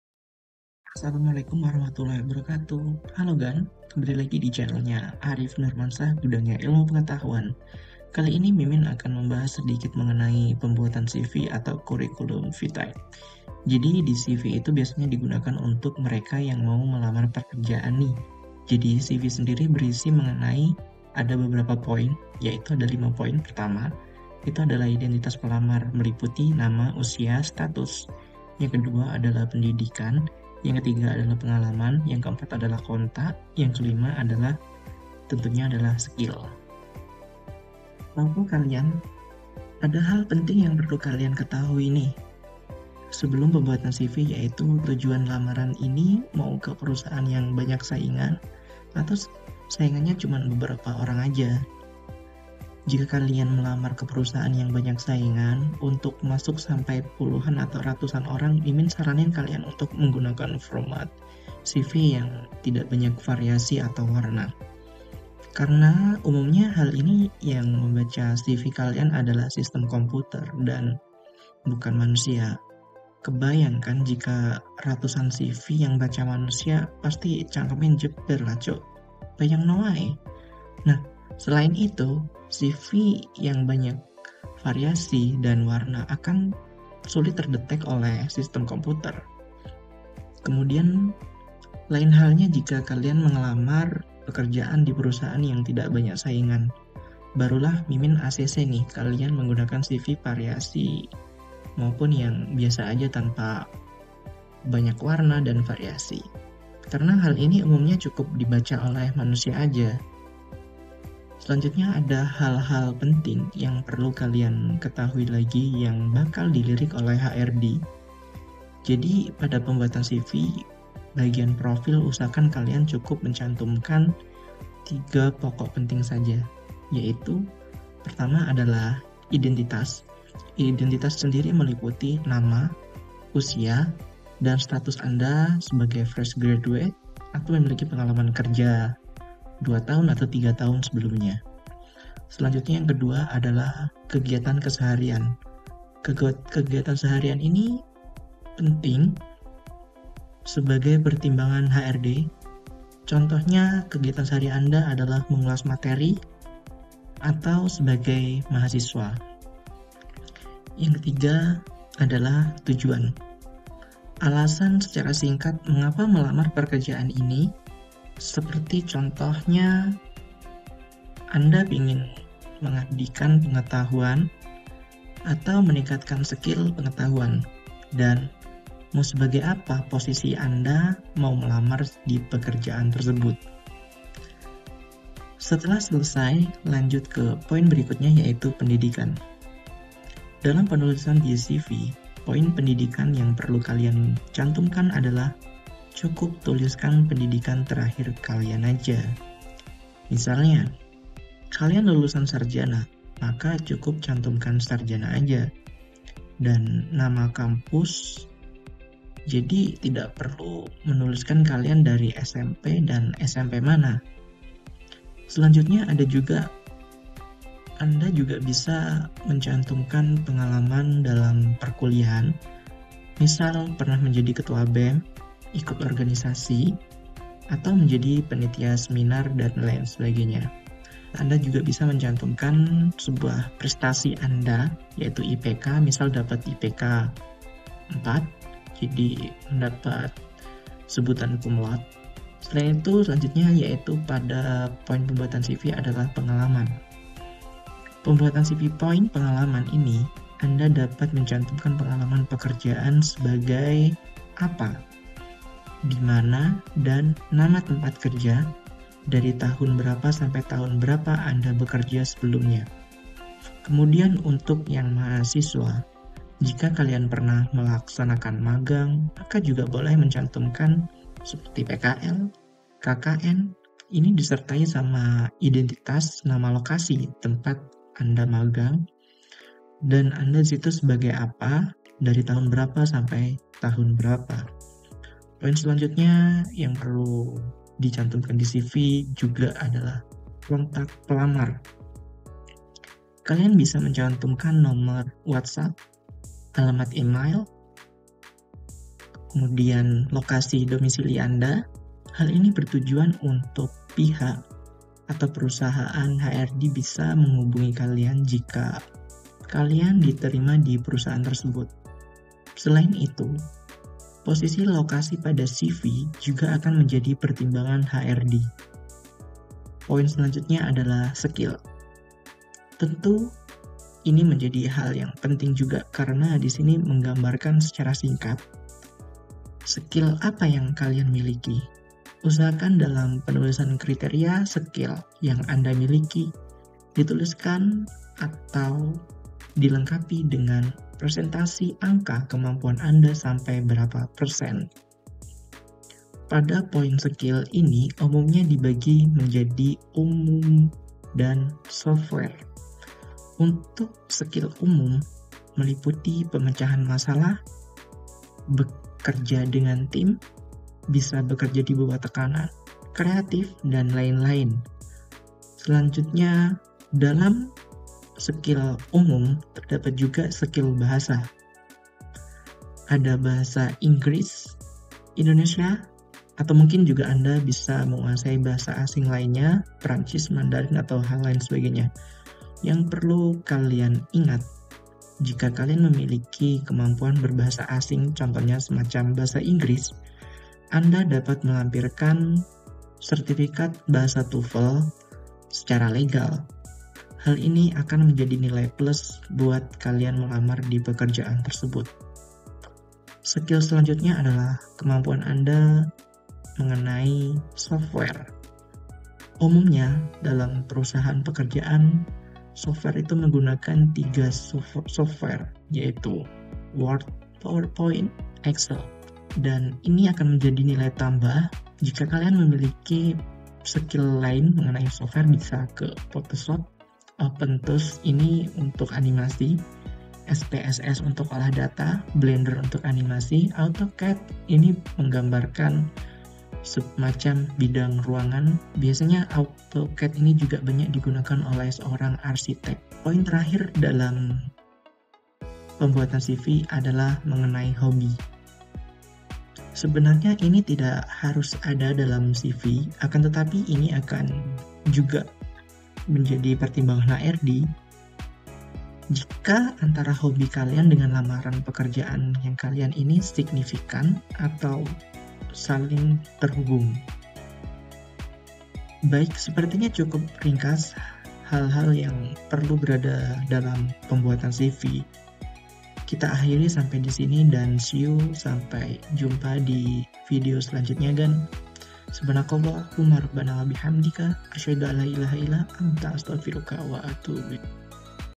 Pokok Penting dalam Pembuatan CV (Curriculum Vitae)
CV atau kata lain Curriculum Vitae adalah bentuk gambaran sekaligus perkenalan mengenai diri seseorang. CV umumnya digunakan untuk melamar pekerjaan. akan tetapi tidak semua CV dapat tembus atau diterimannya seseorang masuk dalam pekerjaan tersebut. nah pembahasa Youtube kali ini membahas pokok-pokok penting yang wajib diikutsertakan dalam pembuatan CV.